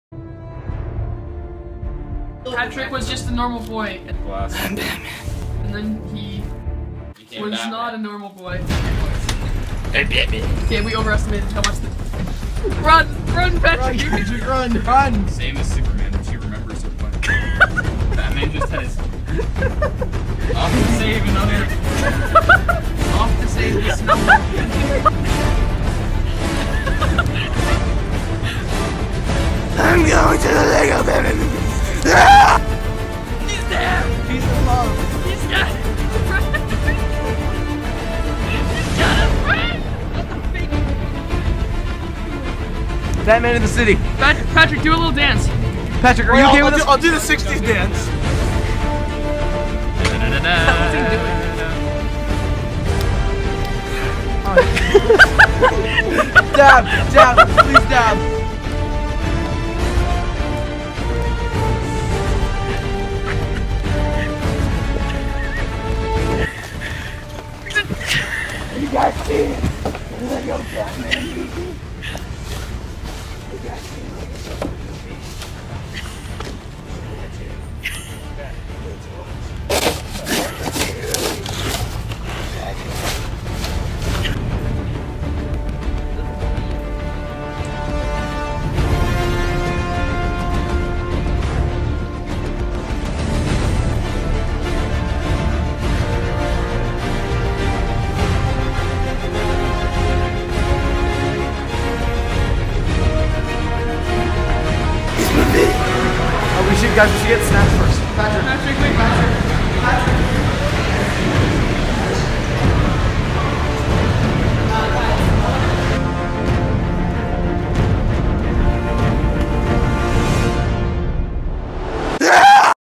Batman Social Experiment (gone wrong) (gone sexual) (gone with the wind) (NOT CLICKBAIT)
On our way to the Lego Batman movie, Patrick gets passionate.